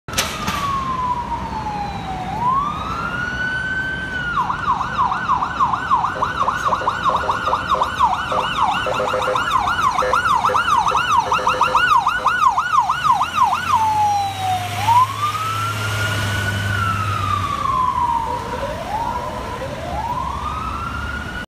NYU LANGONE MEDICAL CENTER EMS AMBULANCE RESPONDING ON W. 42ND ST. IN HELL'S KITCHEN, MANHATTAN.
HERE YOU WILL SEE A NYU LANGONE MEDICAL CENTER EMS AMBULANCE RESPONDING ON WEST 42ND STREET IN THE HELL'S KITCHEN AREA OF MANHATTAN IN NEW YORK CITY.  A SPECIAL SHOUT OUT TO MY FRIEND JULIAN STEINHARDT WHO IS VISITING THE CITY AND FIRE BUFFING WITH ME ON THE MEAN STREETS OF NEW YORK CITY.  PLEASE BE SURE TO CHECK OUT HIS CHANNEL FOR SOME NICE EUROPEAN EMERGENCY SERVICES VIDEOS & WHATEVER HE FILMS HERE WITH ME!